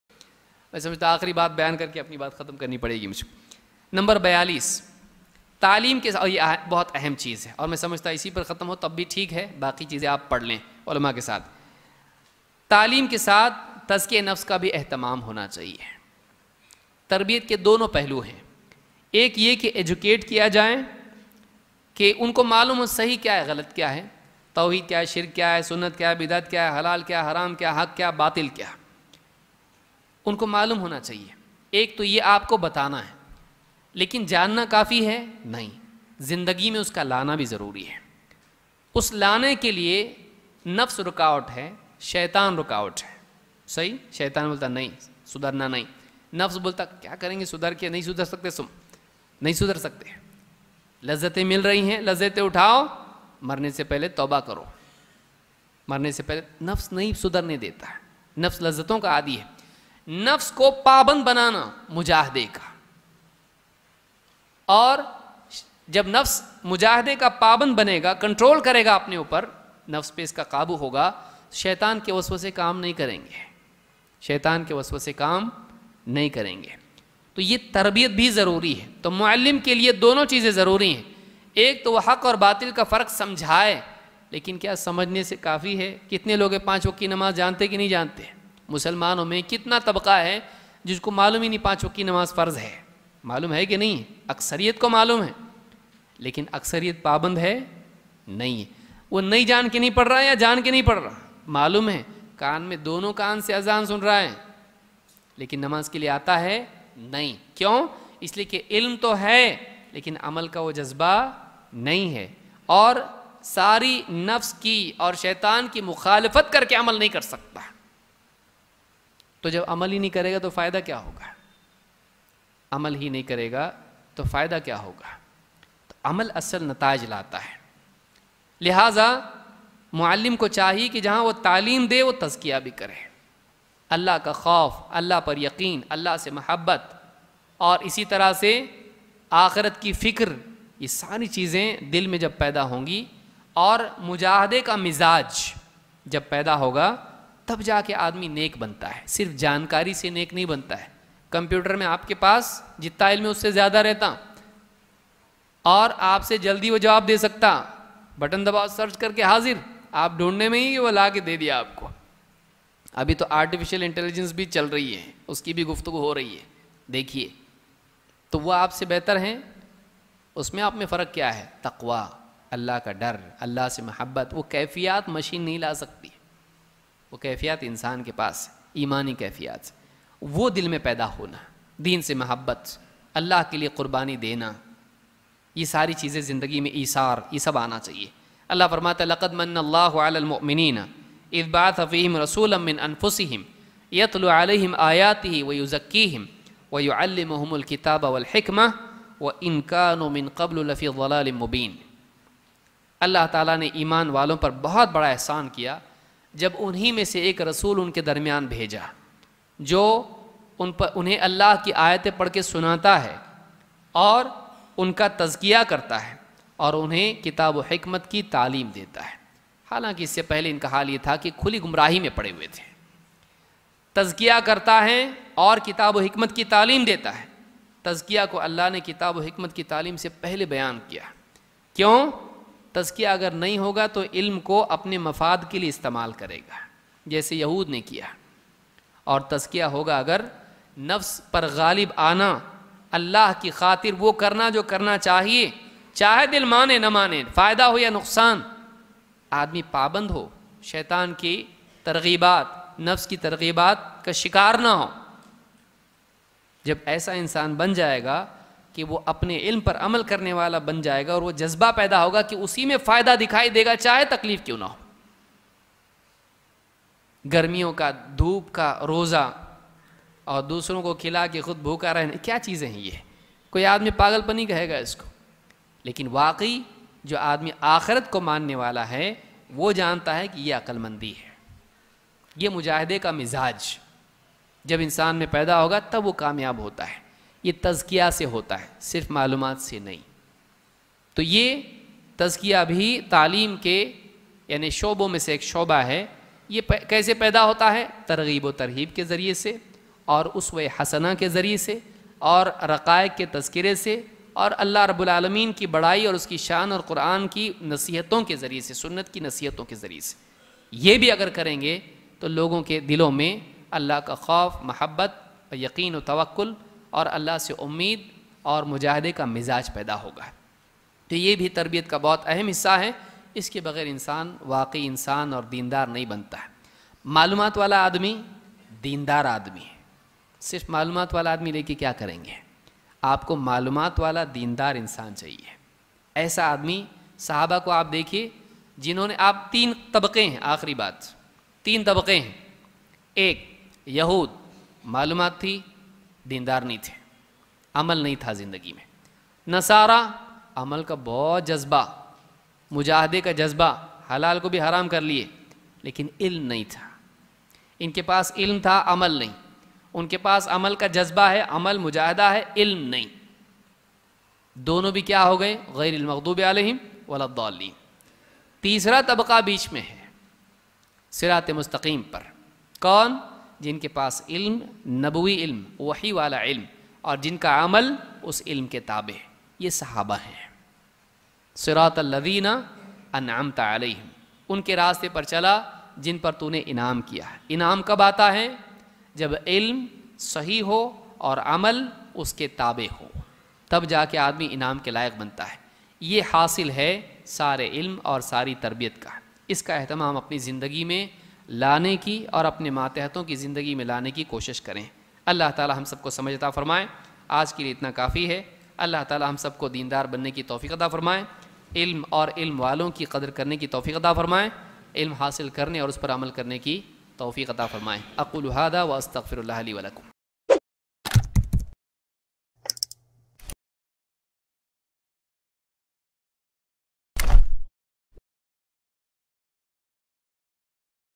42. Taleem Ke Saath Tazkiyah e Nafs Ka Ehtimaam | Shaykh Abu Zaid Zameer
Series: Talab-e-Ilm
(Ilm ki Fazeelat wa Aadaab se  mutalliq 42 abwaab)

Part 42/42: Taleem Ke Saath Tazkiyah e Nafs Ka Ehtimaam

٠٠٠٠٠٠٠٠٠٠٠٠٠٠٠٠٠٠٠٠٠٠٠٠٠٠٠٠٠٠٠٠٠٠٠٠٠٠٠٠